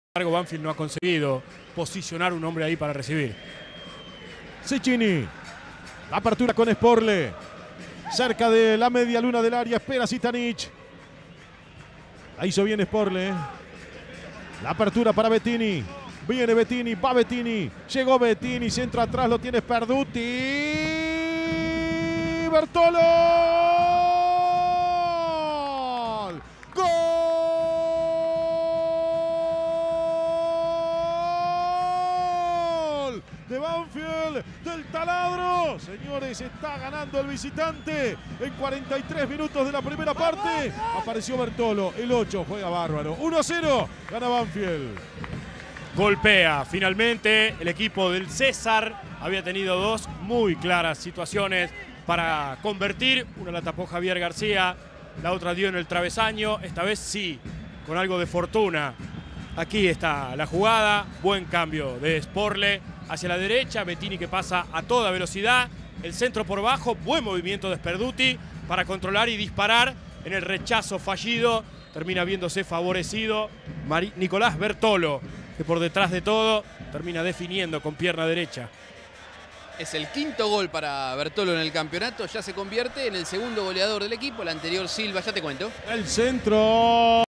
Torneo de la Independencia - Fecha 22 - Banfield 1 - Tigre 0 - Gol de Nicolás Bertolo
Por la 22º fecha del Torneo, Tigre recibió a Banfield en un partido de presentes distintos. El local, que en la fecha pasada perdió 2 a 1 ante Colón, está jugando un mal torneo y naufraga en el fondo de la tabla. El Taladro, de buen campeonato, sueña con acercarse a la punta y lucha por una plaza para la Libertadores.

A los 43 minutos del primer tiempo El Taladro armó una buena jugada por derecha y Nicolás Bertolo definió entrando por el segundo palo.

Partido emitido el 29-04-17